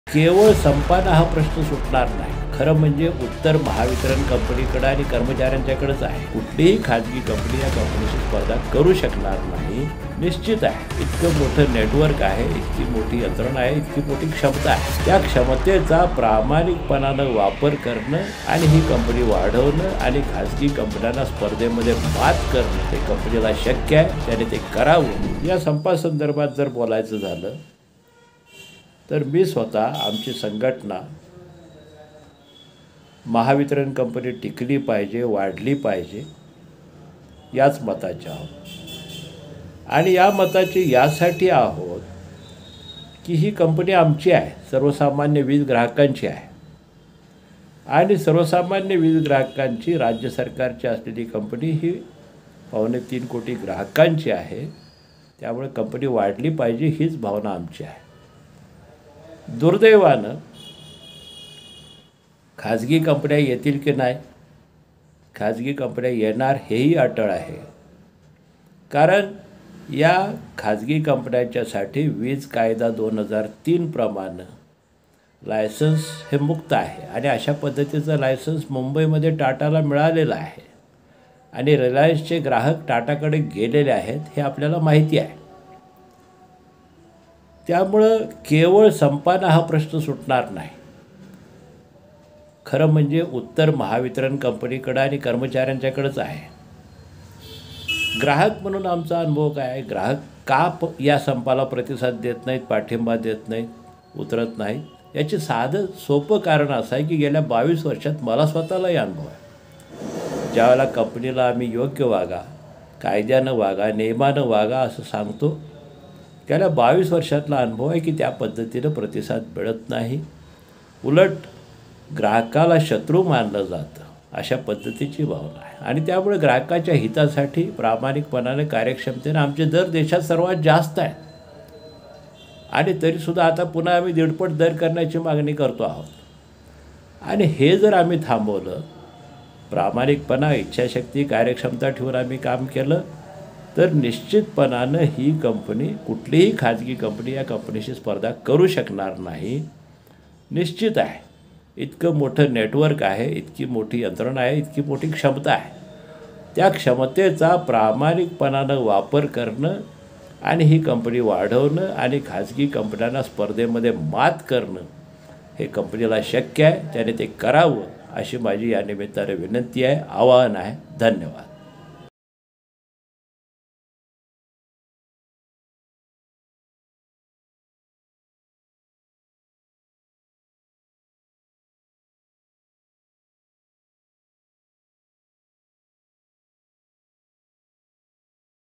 Pratap Hogade : महावितरण कंपनी टिकली पाहिजे...|LetsUpp Marathi
महाराष्ट्र वीज ग्राहक संघटनेचे अध्यक्ष प्रताप होगाडे यांनी कर्मचाऱ्यांच्या संपावर आपली भूमिका मांडली. महावितरण कंपनी टिकली आणि वाढली पाहिजे, असं मत त्यांनी व्यक्त केलं.